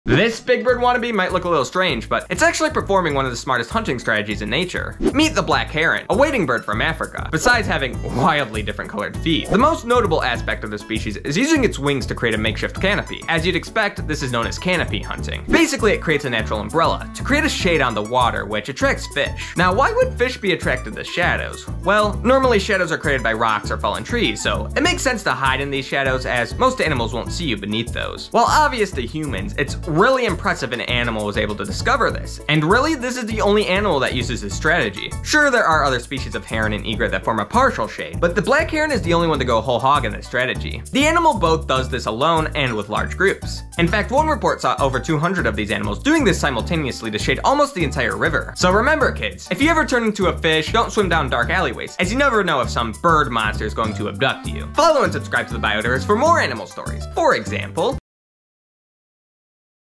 The 🤯🧠SMARTEST Hunting Strategy in Nature!! (Canopy Hunting)
Animals use all sorts of hunting methods  in the wild, but the Black Heron might have the 🧠BIGGEST BRAIN🧠 strategy of them all. Watch this short to learn about the GENIUS of canopy hunting!